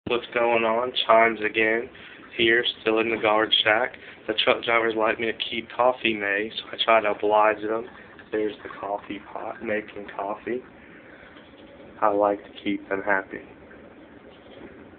Makin Coffee
The live haul drivers like to have coffee made, it helps keep them awake as they drive at night.